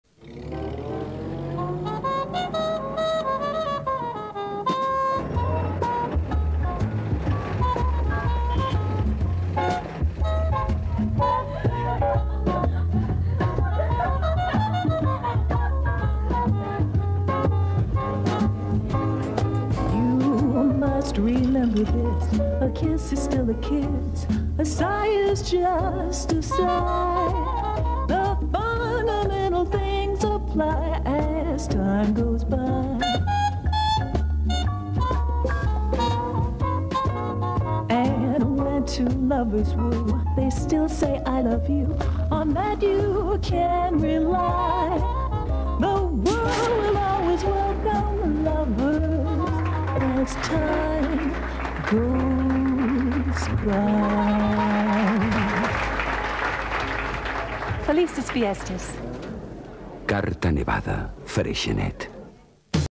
Anuncio - Freixenet (Kim Basinger)
Anuncio de "Freixenet" protagonizado por Kim Basinger emitido en 1993 en un bloque de anuncios en la emision de "La Jungla de Cristal" por TVE 1 .